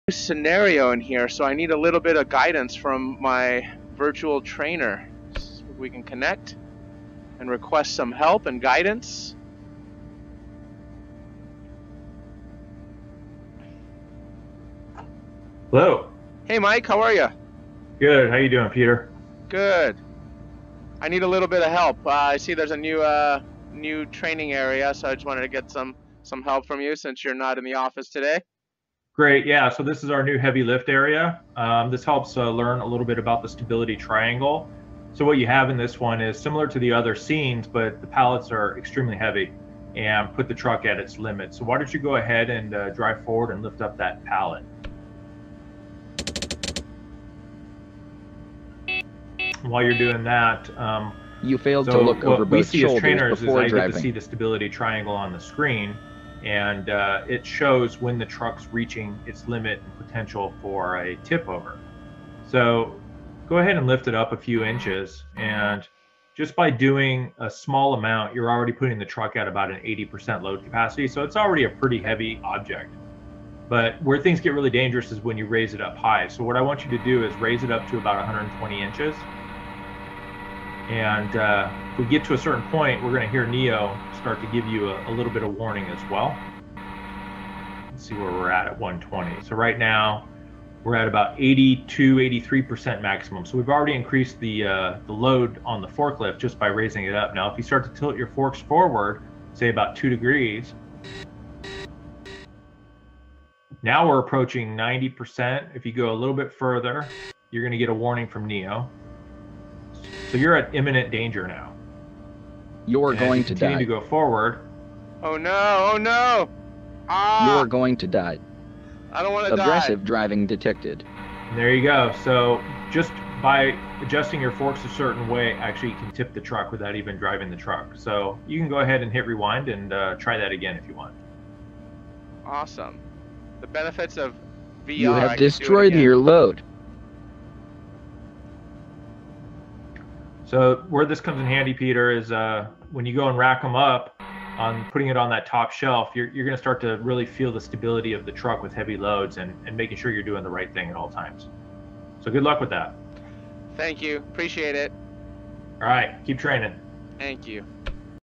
Real-Forklift Virtual Reality Simulator - Remote Trainer Connect & Stability Triangle
In today's world we are facing many limitations that are hampering the training, development and evaluation of our skilled workforce. COVID-19 has accelerated the new digital divide. While office workers were able to work remote and continue with their professional development with online classes and training, the skilled labor workforce had to increase their productivity and sacrifice their professional development.

Generally, skilled positions such as a forklift operator require small group classroom based instructions followed by hands-on one-on-one practical exercises and evaluation.

In 2018, Really-Virtual introduced the Real-Forklift, a virtual reality forklift simulator that provided a near real training equivalent never before available. Companies immediately realized the benefits that VR provides, such as reduced risk, increased hands-on time, reduced space and equipment requirements.

However, with COVID-19, the divide continued to grow. Trainers were limited, classrooms were closed and the operators were asked to do more.

Today, that is all about to change with the introduction of Remote Trainer Connect (RTC). RTC is the Zoom for forklift operator training. Now, operators can continue to train using our forklift simulator solution and still have the benefit of human guided training from experienced trainers from anywhere without concern or need for social distancing.

RTC, allows an operator to request assistance and from anywhere. Their trainers will be notified via their Command Post or Observation Post application. Trainers can then connect remotely using their mobile device or PC. Trainers will have a first-person view of the operator while the operator will see their trainer on their virtual tablet with crystal clear two-way voice communications